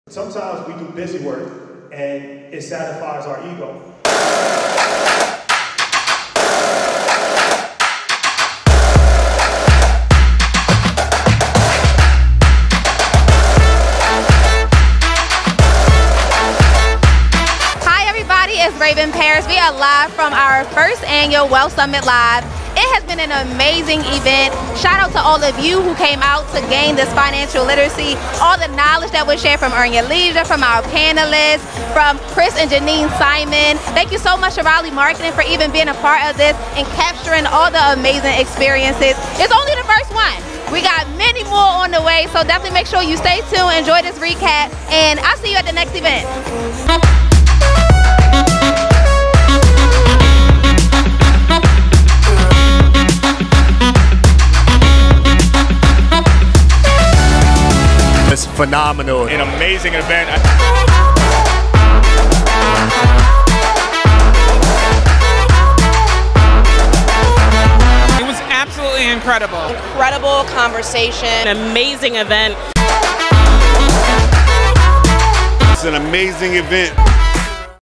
Earn Your Leisure came to Morgan State University
DMV elites came together on July 8th for the most entertaining, educational, and fascinating financial literacy event that has ever come to the DMV featuring the stars of the hugely popular podcast, Earn Your Leisure, powered by Raven Paris and Resilient Tony. Some of the top innovators and most successful business owners from the area were at this event including some of the wealthiest realtors, credit consultants, dispensary owners, and stock traders.

Thank you Raleigh Marketing for this fire recap! @photographywithraleigh